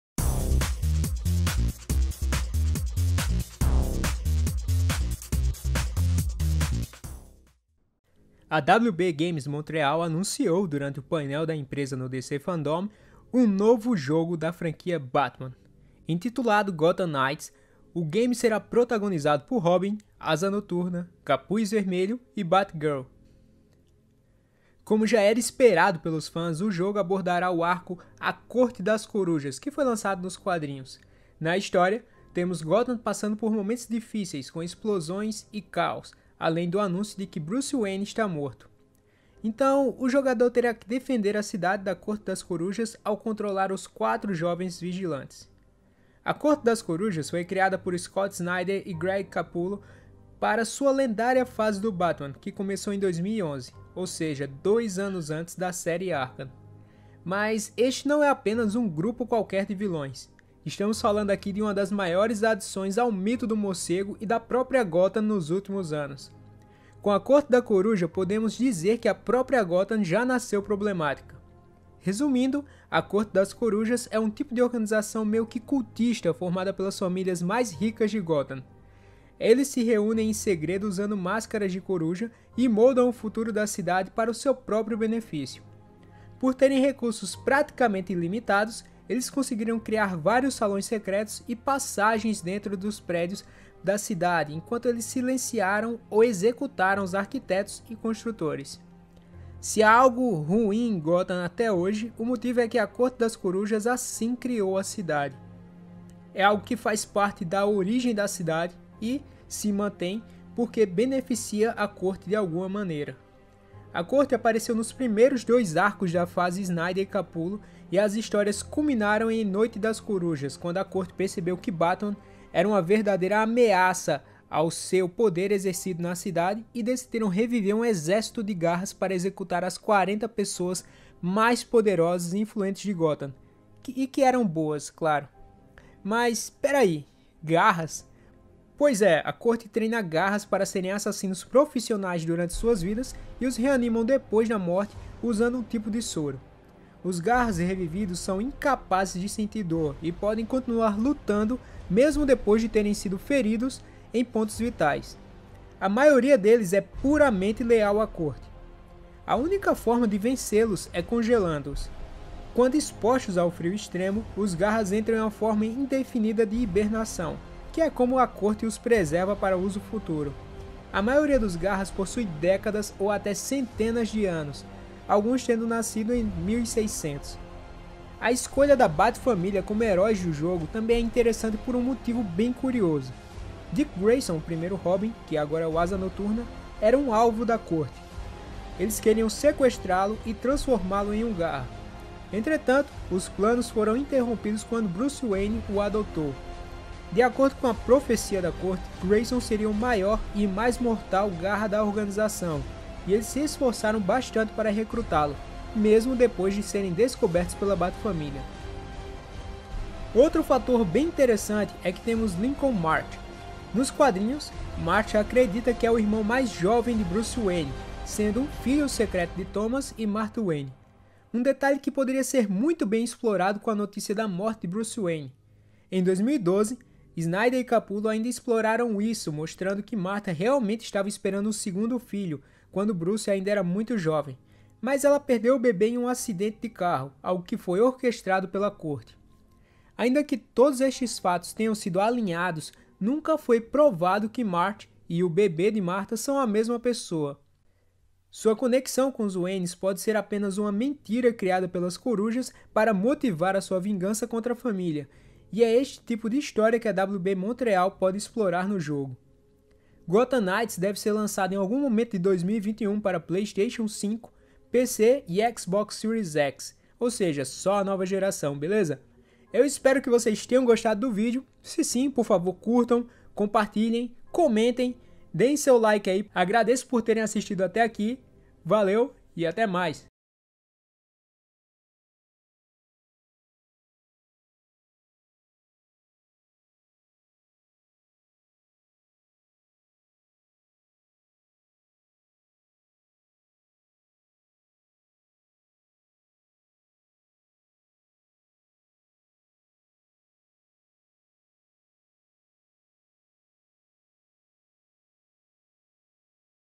O QUE É A CORTE DAS CORUJAS? CONHEÇA OS VILÕES DE GOTHAM KNIGHTS
A WB Games Montreal anunciou, durante o painel da empresa no DC FanDome, o novo jogo da franquia ‘Batman’. Intitulado Gotham Knights, o game será protagonizado por Robin, Asa Noturna, Capuz Vermelho e Batgirl. Como já era esperado pelos fãs, o jogo abordará o arco da HQ da Corte das Corujas.

A Corte das Corujas foi criada por Scott Snyder e Greg Capullo para sua lendária fase do Batman, que começou em 2011, ou seja, 2 anos antes da série Arkham. Mas este não é apenas um grupo qualquer de vilões. Estamos falando aqui de uma das maiores adições ao mito do Morcego e da própria Gotham nos últimos anos. Com a Corte das Corujas, podemos dizer que a própria Gotham já nasceu problemática.

Resumindo, a Corte das Corujas é um tipo de organização meio que cultista formada pelas famílias mais ricas de Gotham. Eles se reúnem em segredo usando máscaras de corujas e moldam o futuro da cidade para seu próprio benefício. Por terem recursos praticamente ilimitados, eles conseguiram criar vários salões secretos e passagens dentro dos prédios da cidade, enquanto eles silenciaram ou executaram os arquitetos e construtores.
Se há algo ruim em Gotham até hoje, o motivo é que a Corte das Corujas assim criou a cidade. É algo que faz parte da raíz da cidade e, claro, porque beneficia a corte de alguma maneira.

A corte apareceu nos primeiros dois arcos da fase de Snyder e Capullo e as histórias culminaram em Noite das Corujas, quando a Corte percebeu que Batman era uma verdadeira ameaça ao seu poder exercido na cidade e decidiram reviver um exército de Garras para executar as 40 pessoas mais poderosas e influentes de Gotham (que eram boas, claro).

Mas espera aí… Garras?

Pois é, a Corte treina Garras para serem assassinos profissionais durante suas vidas e os reanimam depois de morte usando um tipo de soro. os Garras revividos são incapazes de sentir dor e podem continuar lutando mesmo depois de terem sido feridos em pontos vitais. A maioria deles é puramente leal à corte.

A única forma de vencê-los é congelando-os. Quando expostos ao frio extremo, os Garras entram em uma forma indefinida de hibernação, que é como a Corte os preserva para uso futuro. A maioria dos Garras possui décadas ou até centenas de anos, alguns tendo nascido em 1600.

E a escolha da Batfamília como heróis do jogo também é interessante por um motivo bem curioso: Dick Grayson, o primeiro Robin, era um alvo da Corte. Eles queriam sequestrá-lo e transformá-lo em um Garra. Entretanto, os planos foram interrompidos quando Bruce o adotou. De acordo com uma profecia da Corte, Grayson seria o maior e mais mortal Garra da organização e eles se esforçaram bastante para recrutá-lo mesmo depois de serem descobertos pela batfamília.

E outro fator bem interessante é que temos Lincoln March. Nos quadrinhos, March acredita que é o irmão mais novo de Bruce, sendo um filho secreto de Thomas e Martha Wayne. Um detalhe que poderia ter muito peso com a notícia da morte de Bruce Wayne. Em 2012, Snyder e Capullo ainda exploraram isso, mostrando que Martha realmente estava esperando um segundo filho quando Bruce era muito jovem, mas ela perdeu o bebê em um acidente de carro, algo que foi orquestrado pela Corte.

Ainda que todos estes fatos tenham sido alinhados, nunca foi provado que March e o bebê de Martha são a mesma pessoa. Sua conexão com os Waynes pode ser apenas uma mentira criada pelas Corujas para motivar sua vingança contra a família e este é um tipo de história que a WB Montreal pode explorar.

Gotham Knights deve ser lançado em algum momento de 2021 para PlayStation, PC e Xbox.